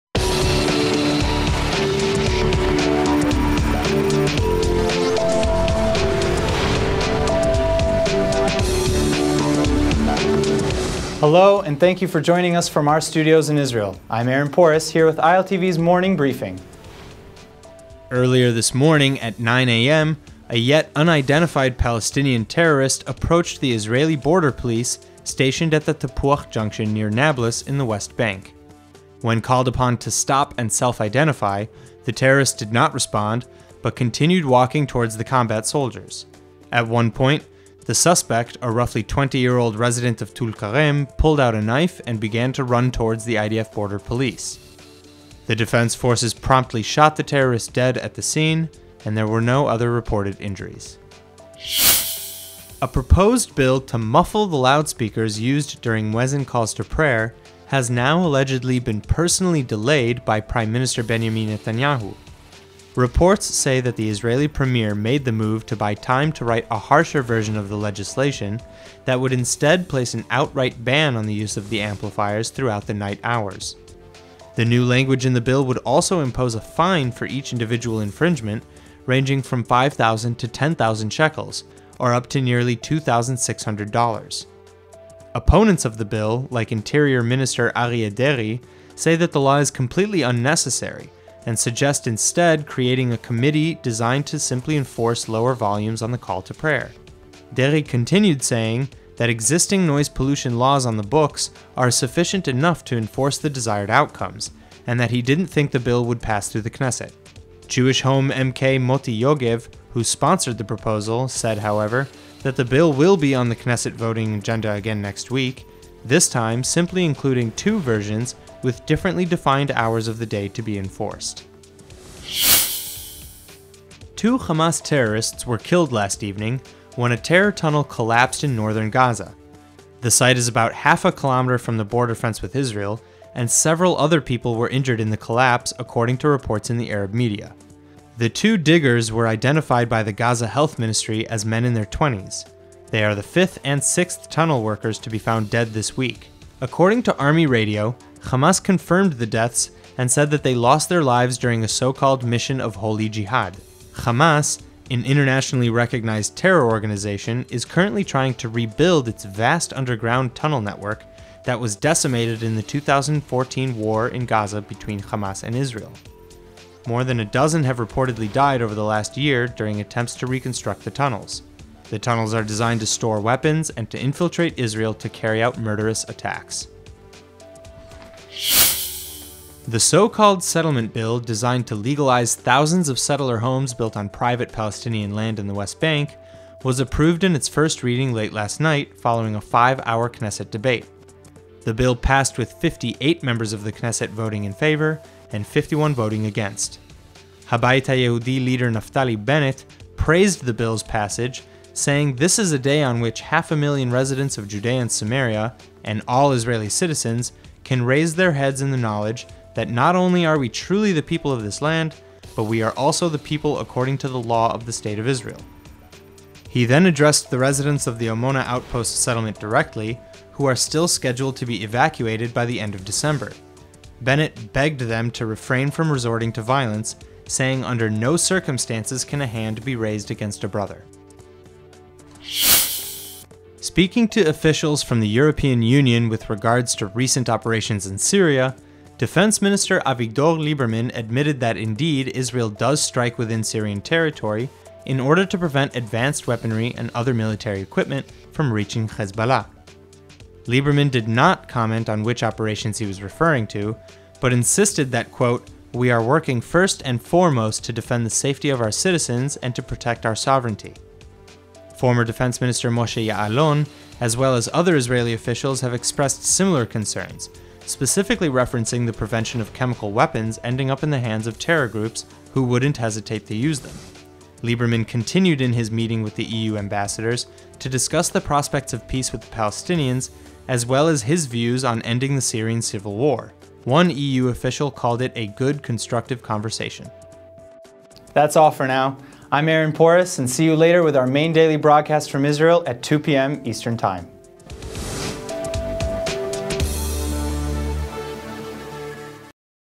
Your Morning News From Israel - December 8, 2016
Terrorist brandishing a knife is shot dead, muezzin call-to-prayer muffling bill benched for now, two Hamas terrorists killed in tunnel collapse, settlement bill passes the first of the three Knesset readings, and Lieberman discusses Syrian strikes with E.U. ambassadors.